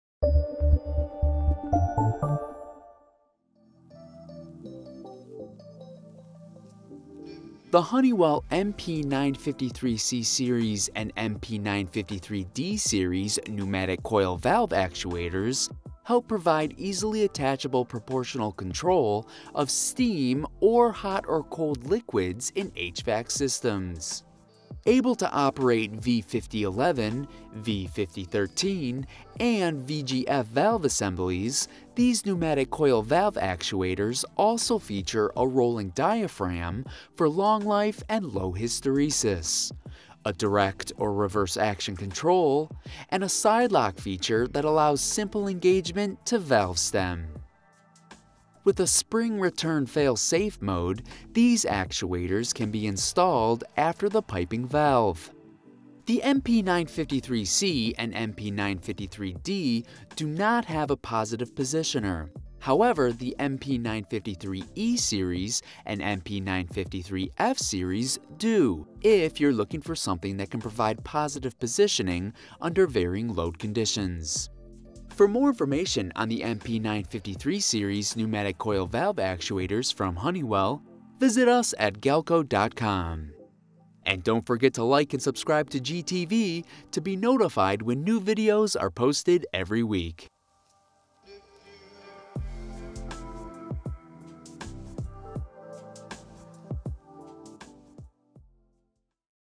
Honeywell's MP953 Series, Pneumatic Coil Valve Actuators
The Honeywell MP953C Series and MP953D Series Pneumatic Coil Valve Actuators from help provide easily attachable proportional control of steam or hot or cold liquids in HVAC systems.
Able to operate V5011, V5013, and VGF valve assemblies, these pneumatic coil valve actuators also feature:
• Rolling diaphragm for long life and low hysteresis
• Direct or reverse-action control
• Slide lock feature that allows simple engagement to valve stem
With a spring return fail safe mode, these actuators can be installed after the piping valve.
The MP953C and MP953D Pneumatic Coil Valve Actuators from Honeywell DO NOT have a positive positioner.
However, the MP953E Series and MP953F Series do- if you are looking for something that can provide positive positioning under varying load conditions.